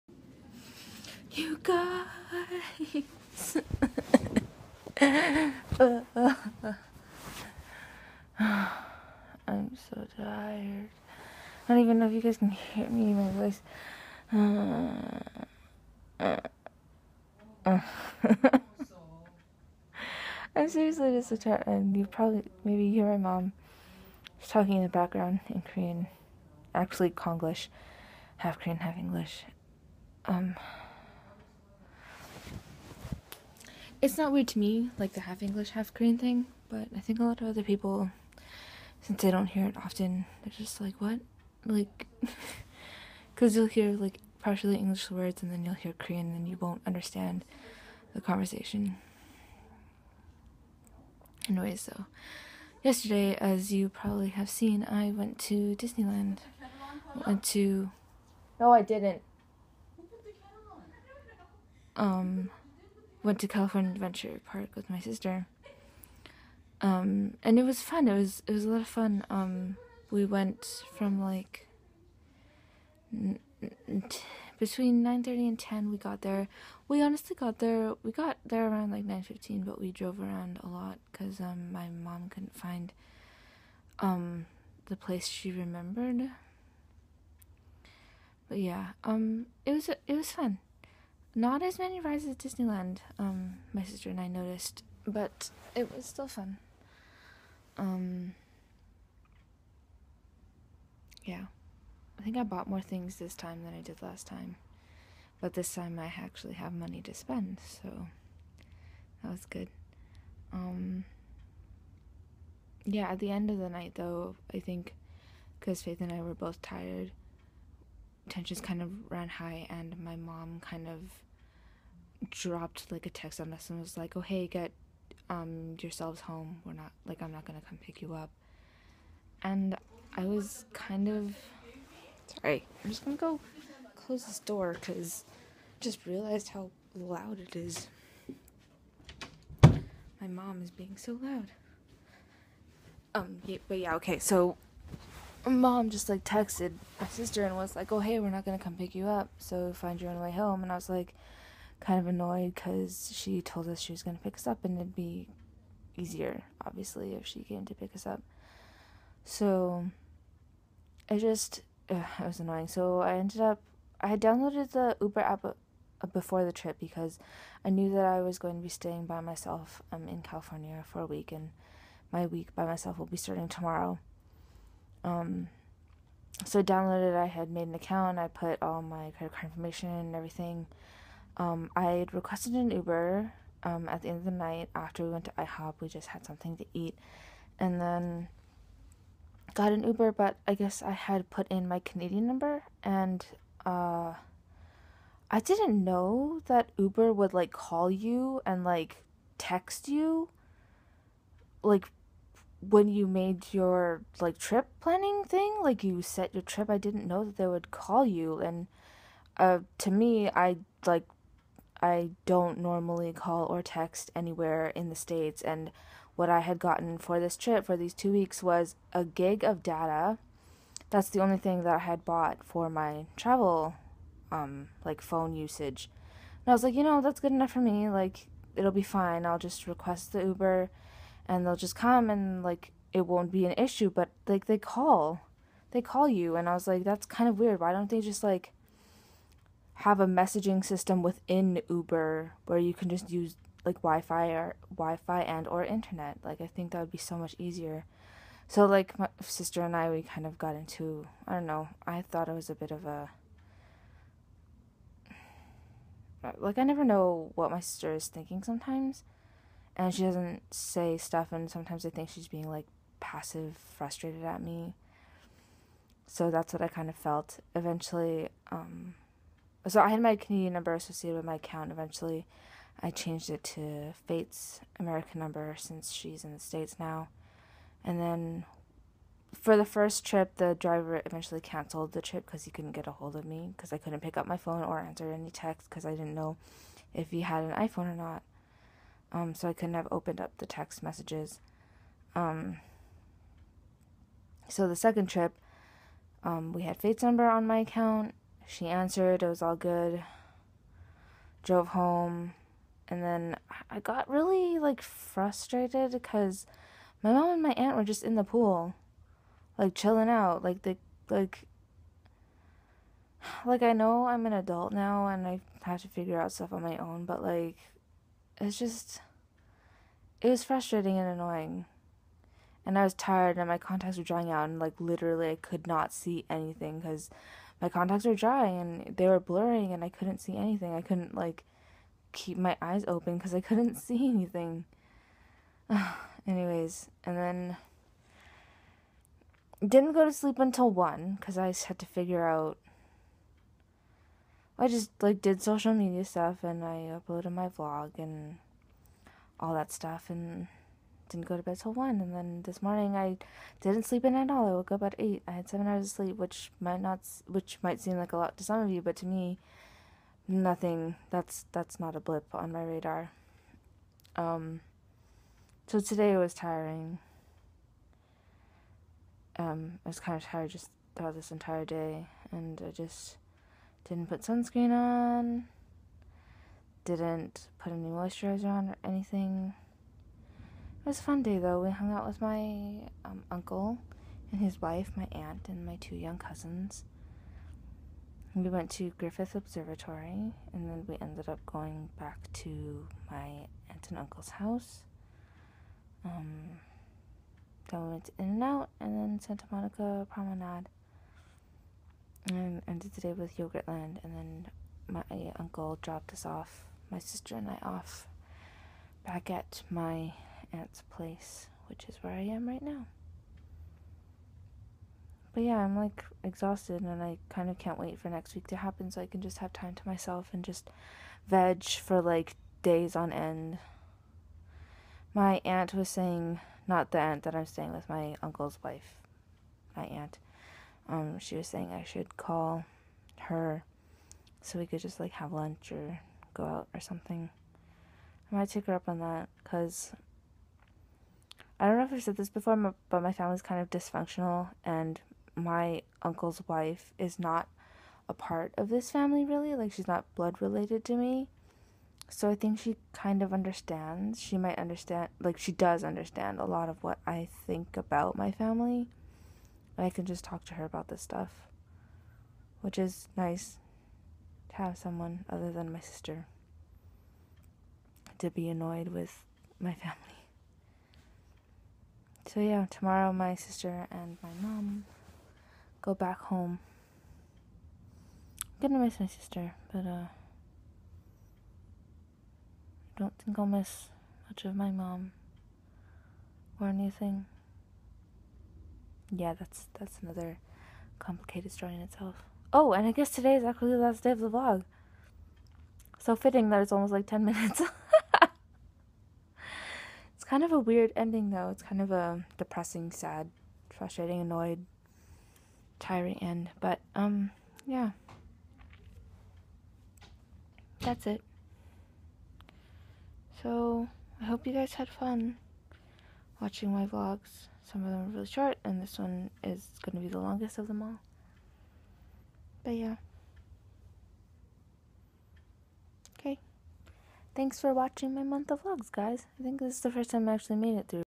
09/30/2016
EDIT: YOU GUYS. I WATCHED IT THROUGH AND IT CUT OFF. UGH! I think it had something to do with how I pressed the stop record button on my phone. WELL. TOO LATE. I think you got the gist of what I was saying at the end though. ^^

The end of an actually completed VEDiS. Got kind of weird...ly emotional.